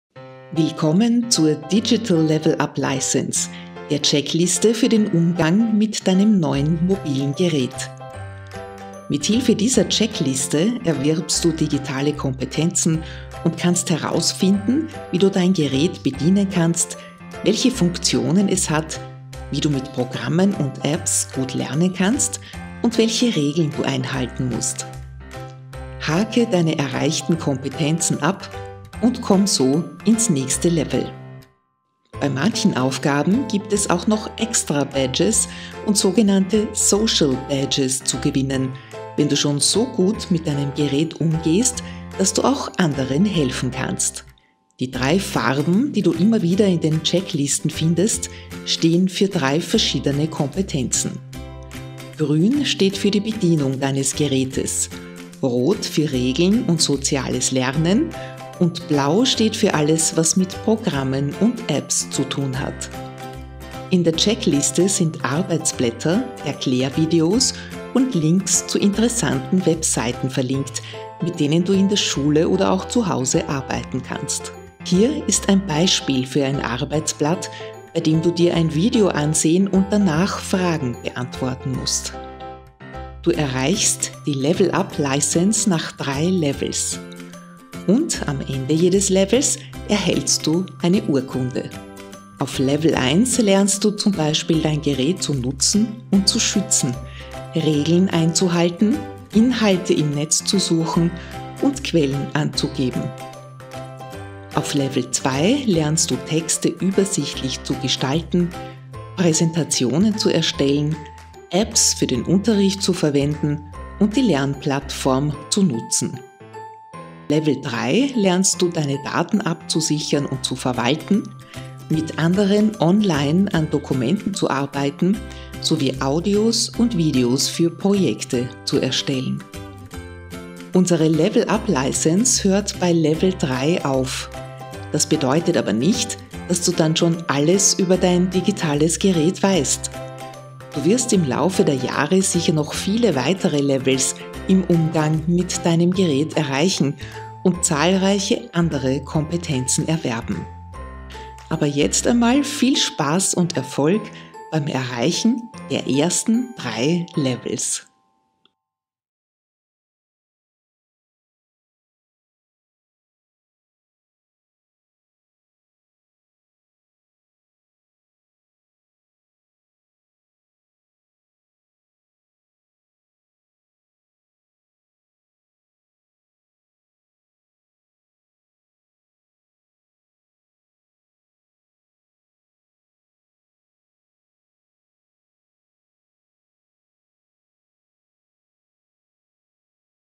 Digital LEVEL-UP Licence: Teaser-Video
Die Digital LEVEL-UP Licence ist ein vom BMBWF gefördertes Projekt im Rahmen des 8-Punkte-Plans, das vom ZLI der PH Wien, der PH Niederösterreich und der KPH Wien/Krems in Zusammenarbeit mit dem OeAD, dem Future Learning Lab Wien, der VPH und der PHeLS im Zeitraum September bis November 2021 umgesetzt wurde.
Die gratis zur Verfügung gestellten Checklisten, Arbeitsmaterialien und Links der LEVEL-UP Licence bieten Hilfestellungen und Impulse für einen kompetenzorientierten Einsatz der Geräte im Unterricht. Schüler:innen können erreichte Kompetenzen in Checklisten abhaken und kommen so ins nächste Level.